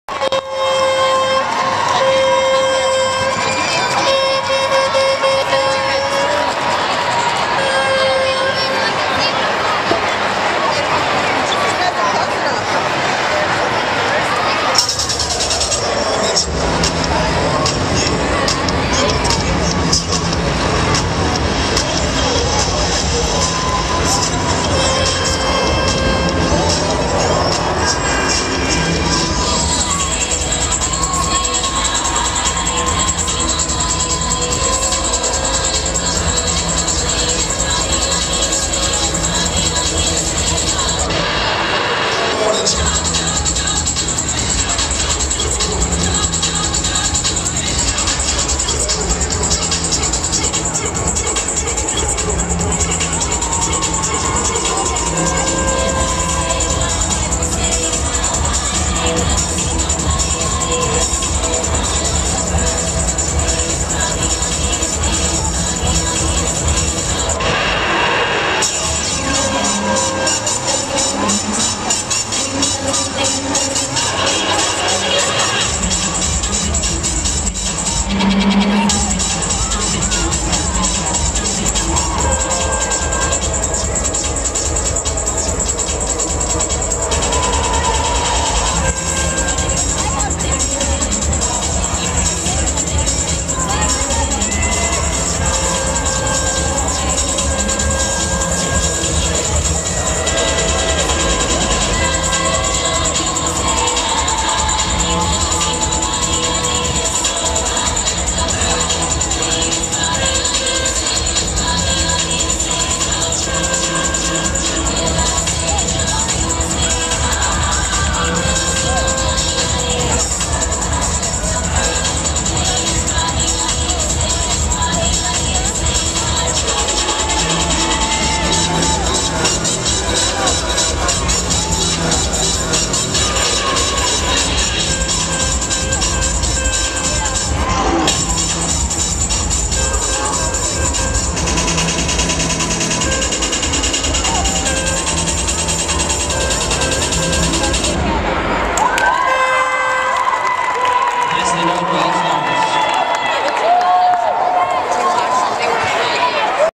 WCCA at NavyPier 2010
Will-Cook Cheer Association performing at Navy Pier (April 2010)